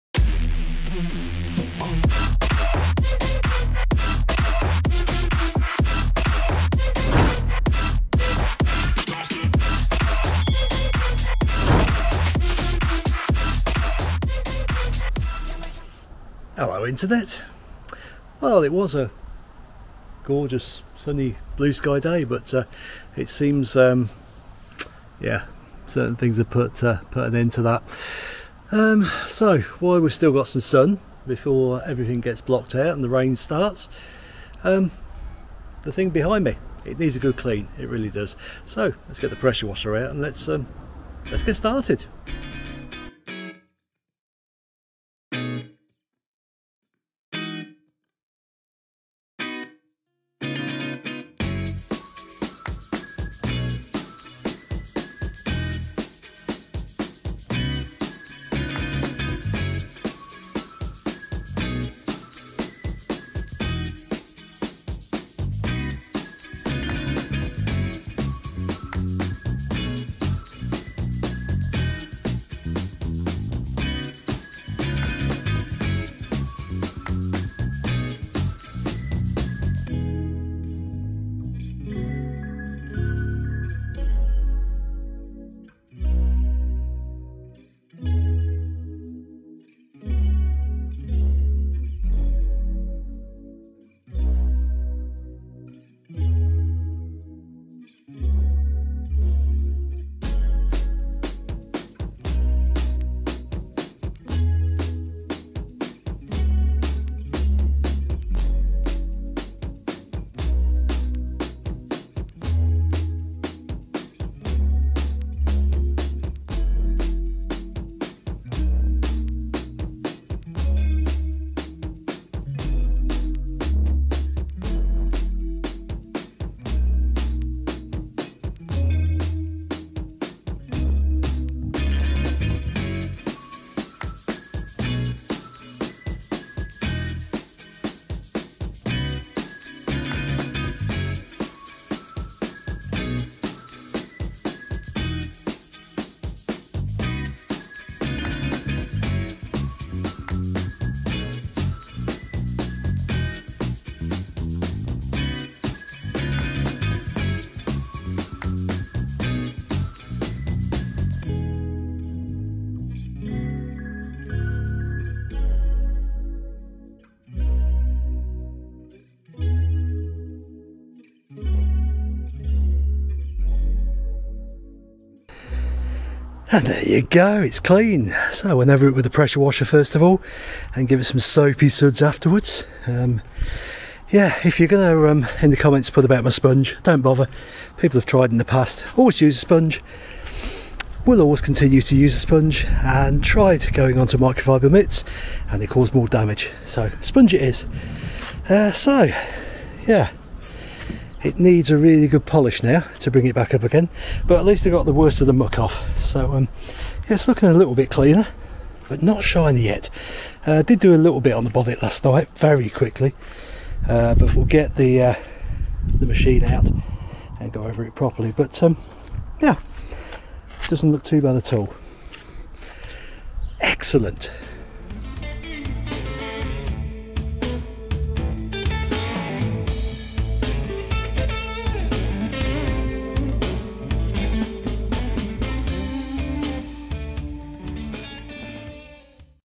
Smart Roadster _ First wash in 2 over years
lets get some of the grime and cobwebs off the old girl.

2004 Smart Roadster.
Smart Roadster recommissioning.
off the road for two years.